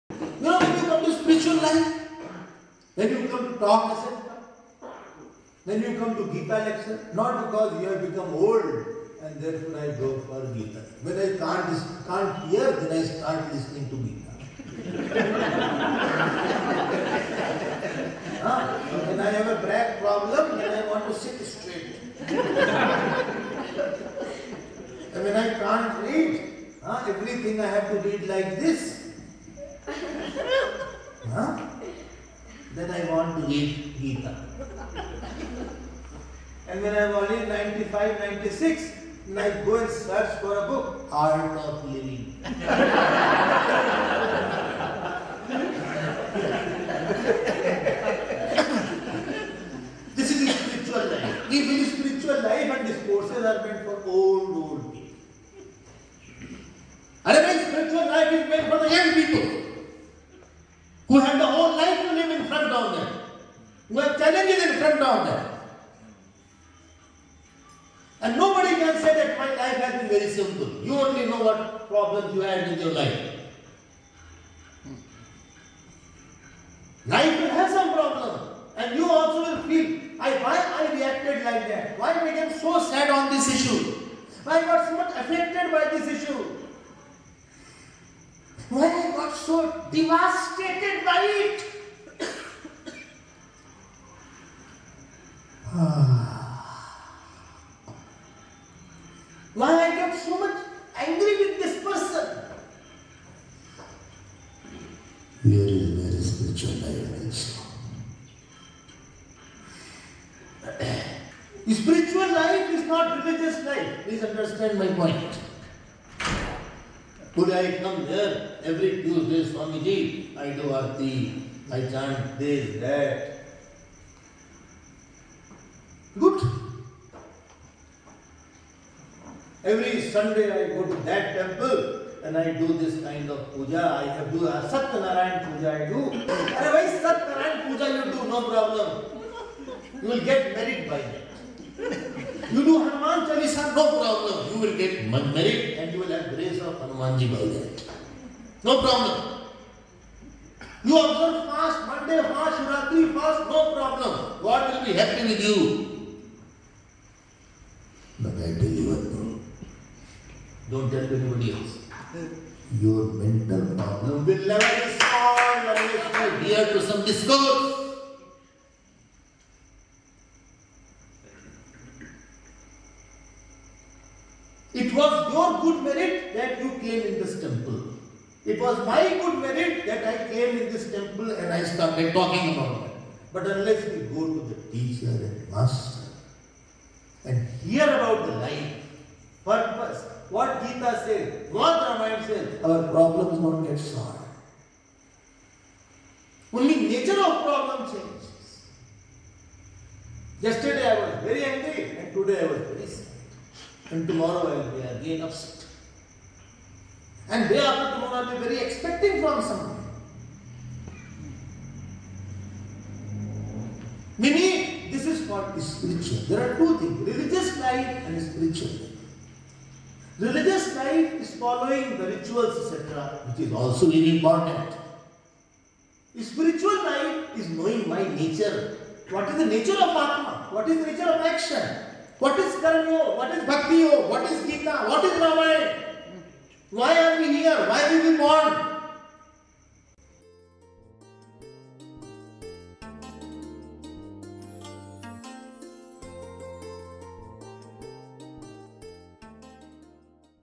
What is Spiritual Life?
s spirituality meant only for the older generation or is it everyone's most urgent and essential need? Watch this video excerpt by Swami Abhedanandaji to know what is the importance of having a spiritual life.